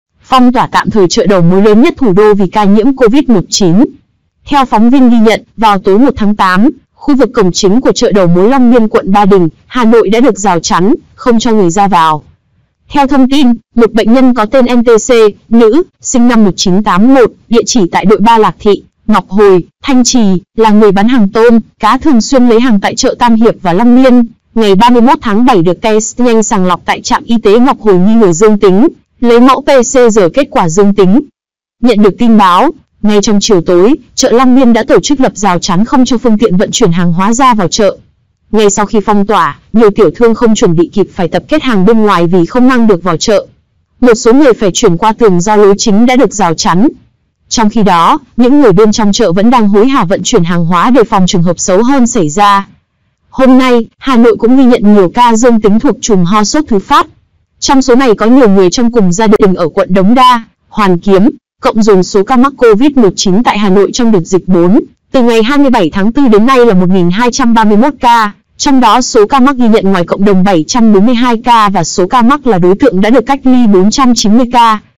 Chợ Long Biên: phong tỏa trong đêm (1/8) vì ca nhiễm COVID-19
Theo phóng viên ghi nhận, vào tối 1/8, khu vực cổng chính của chợ đầu mối Long Biên (quận Ba Đình, Hà Nội) đã được rào chắn, không cho người ra vào.

Theo thông tin, một bệnh nhân có tên N.T.C, nữ, sinh năm 1981, địa chỉ tại Đội 3 Lạc Thị, Ngọc Hồi, Thanh Trì, là người bán hàng tôm, cá thường xuyên lấy hàng tại chợ Tam Hiệp và Long Biên, ngày 31/7 được test nhanh sàng lọc tại Trạm Y tế Ngọc Hồi nghi ngờ dương tính, lấy mẫu PCR kết quả dương tính.